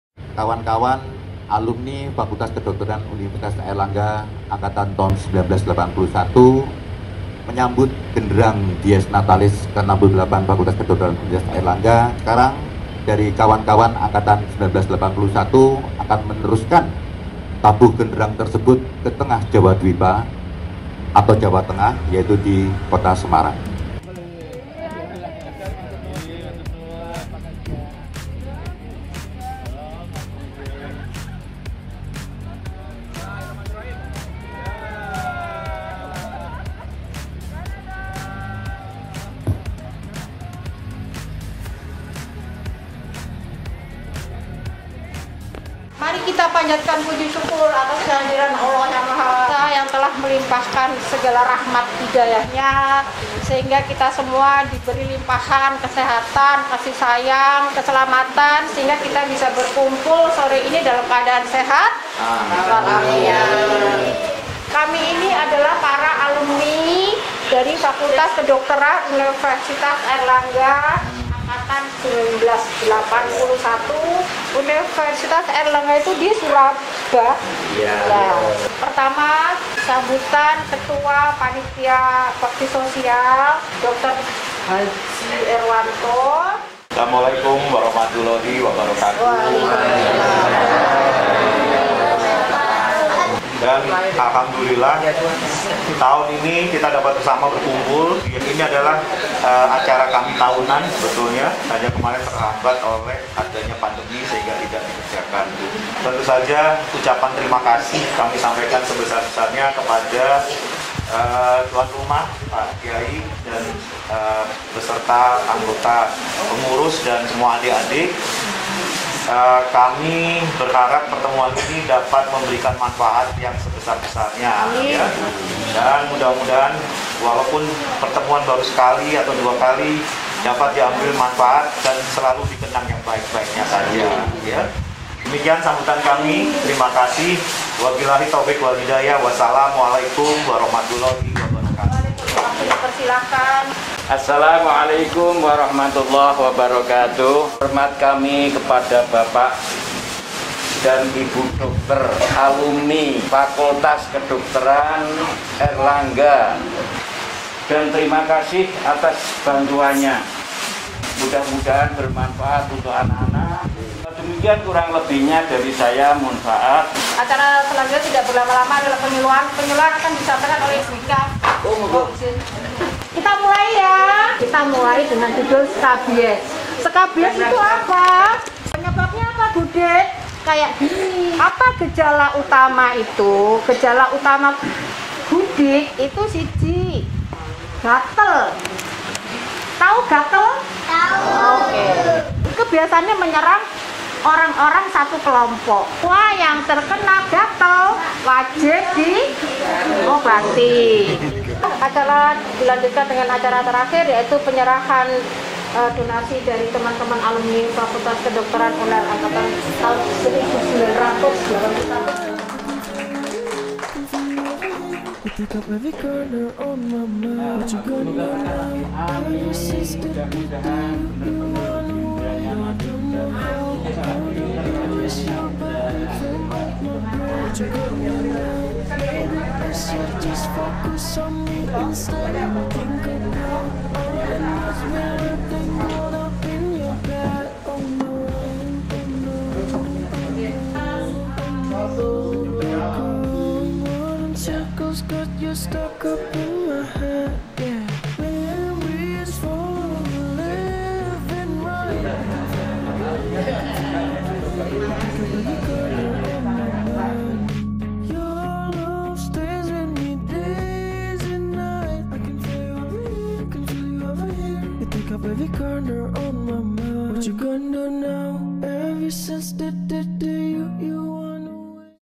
Pengmas FKUA 1981 - Penyuluhan Kesehatan Dan Santunan Anak Yatim di Panti Asuhan Tarbiyatul Semarang
Pengmas FK'81 merupakan pengmas yang pertamakali dilakukan setelah Bapak Dekan FK Unair mencanangkan rangkaian acara Dies Natalis FK Unair ke-68.
Kegiatan ini dilaksanakan pada tanggal 26 Agustus 2022 di Panti Asuhan Tarbiyatul Yatim dengan sasaran hampir 100 anak.
Para peserta mendapatkan penyuluhan kesehatan dengan topik "Scabies", santunan yatim, donasi dan kit personal hygiene.
Pengmas ini dihadiri oleh para alumni FK Unair angkatan 1981.
Terimakasih kepada semua pihak yang telah membantu berjalannya acara.

The Official YouTube Channel of The Faculty of Medicine, Airlangga University - Kanal YouTube Resmi Fakultas Kedokteran Universitas Airlangga